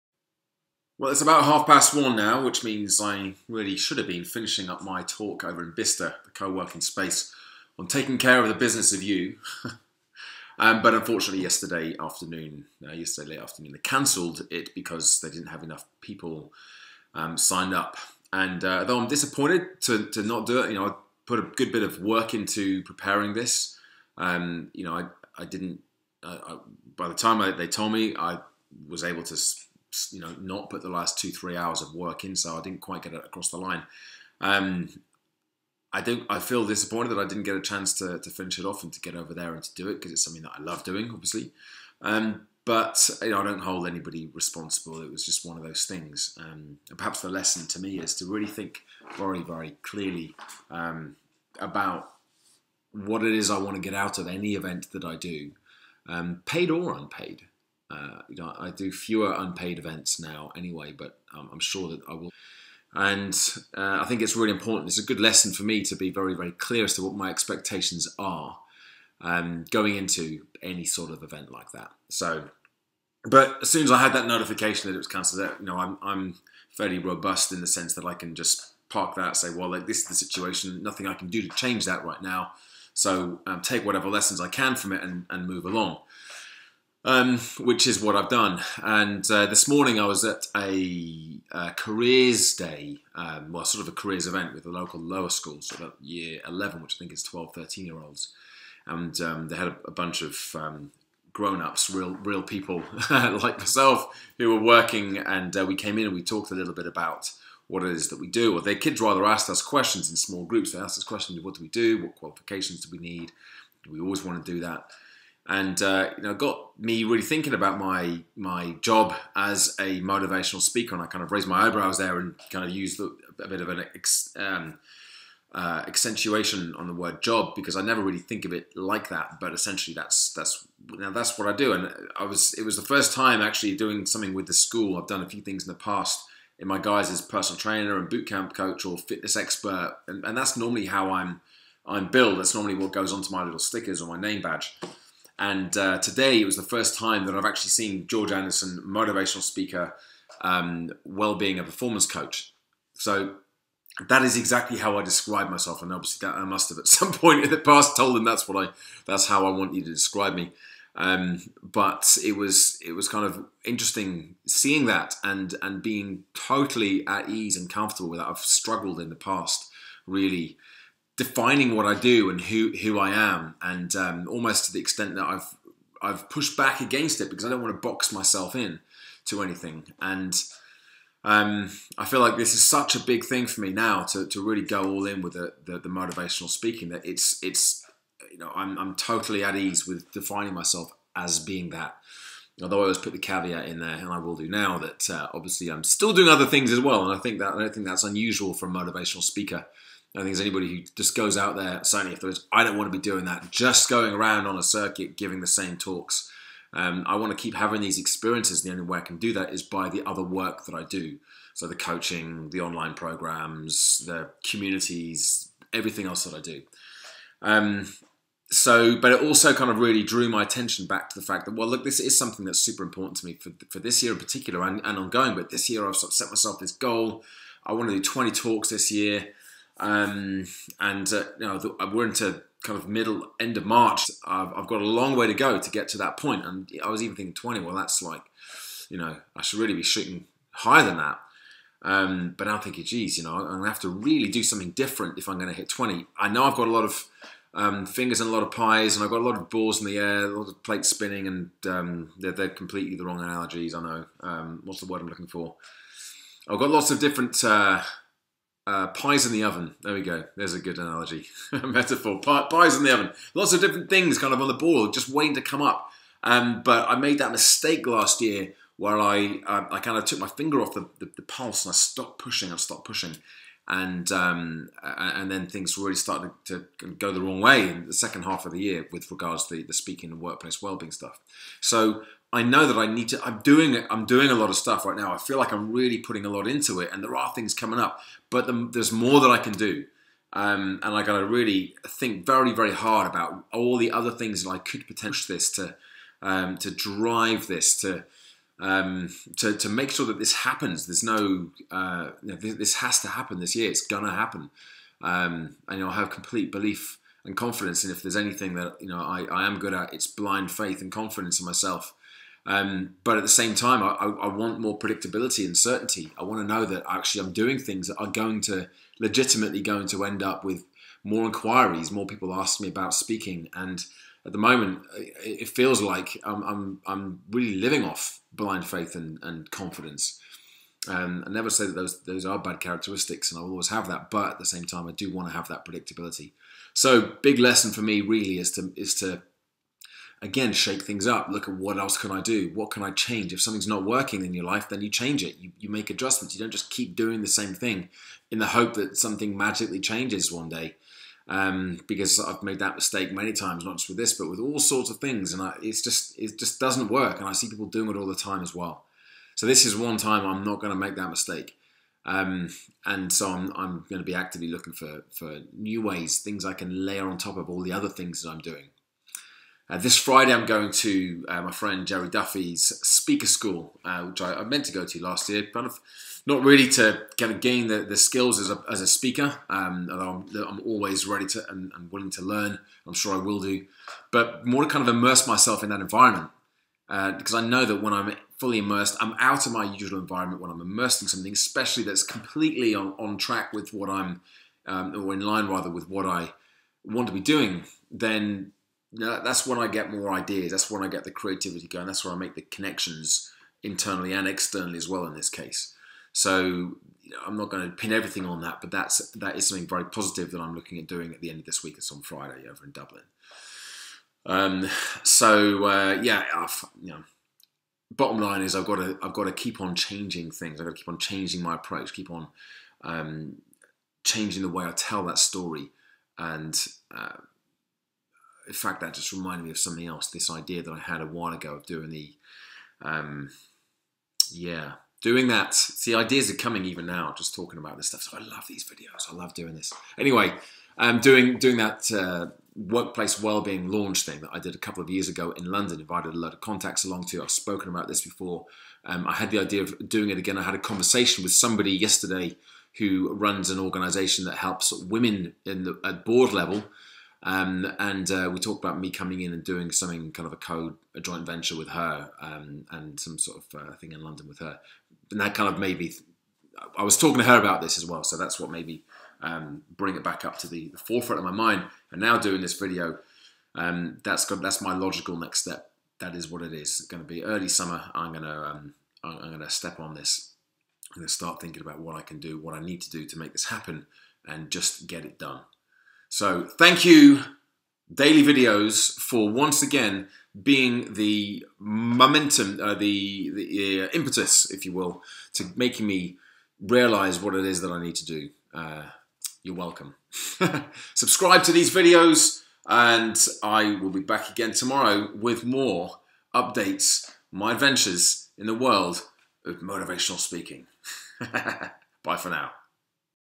GS#079 Talk cancelled
GS#079 Talk cancelled The talk today was cancelled which was disappointing, but no point in dwelling on it. Looking at what I need to do next to step the momentum up another level...